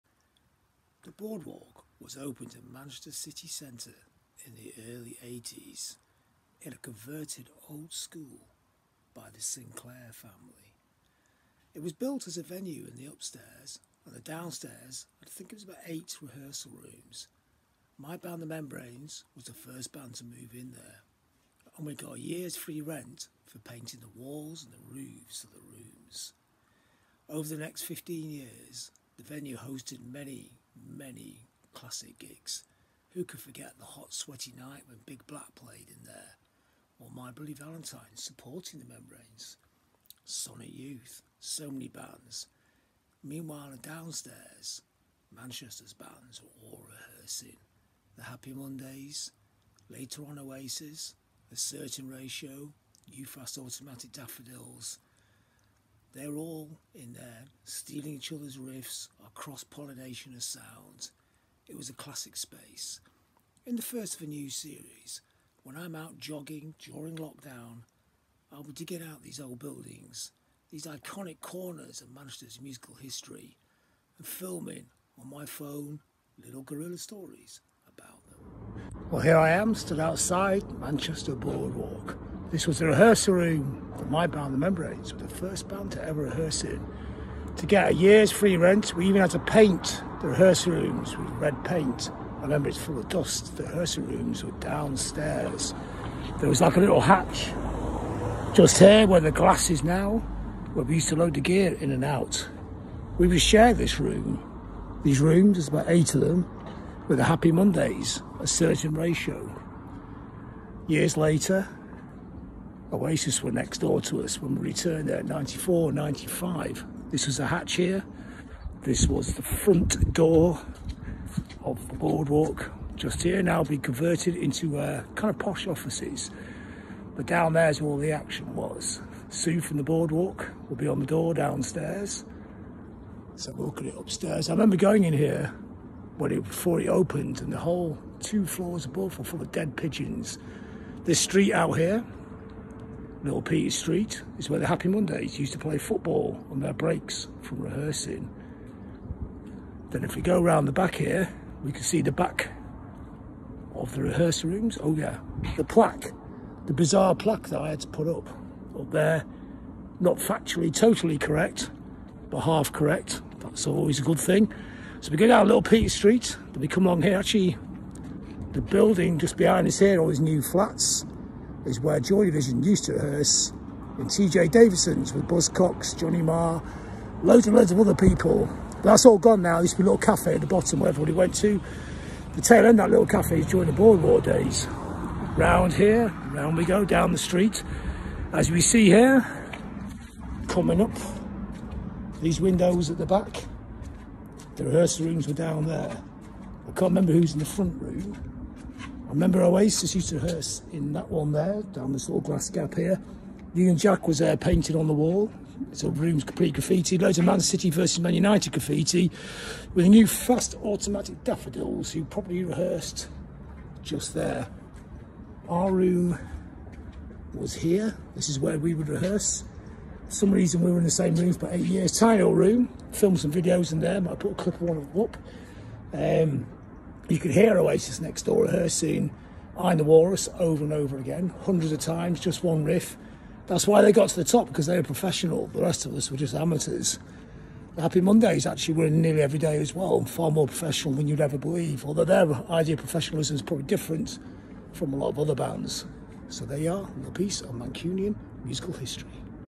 Isolation : John Robb guide to iconic manchester during lockdown : episode one : The Boardwalk
The famous Manchester venue and rehearsal room where Oasis, Happy Mondays, A Certain Ratio, the Membranes and many others rehearsed there. The venue saw classic gigs from Big Black, Oasis, The Happy Mondays, James, Sonic. Youth The Verve play there - it was a key venue from 1984 to 1999.